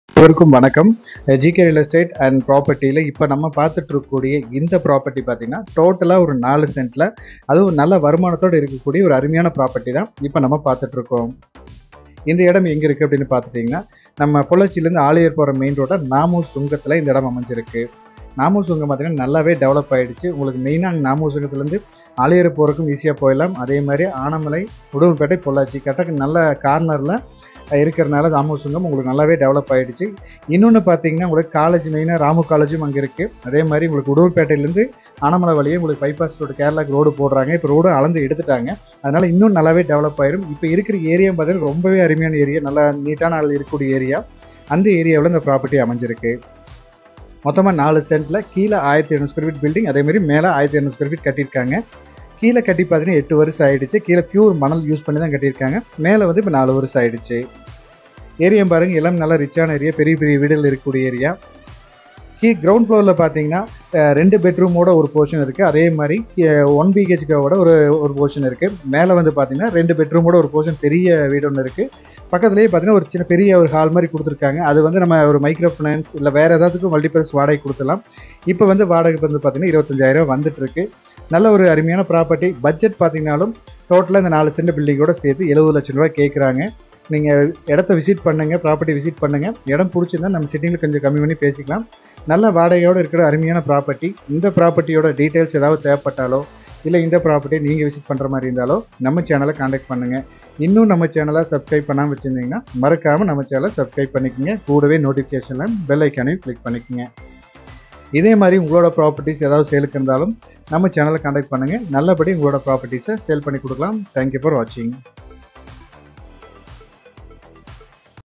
4 cent land with rental income property for sales in Na. mu. sungam
4 cent land area
ground floor
one 2 bhk house
one 1 bhk house
first floor
one big hall
rental income property
Na . mu sungam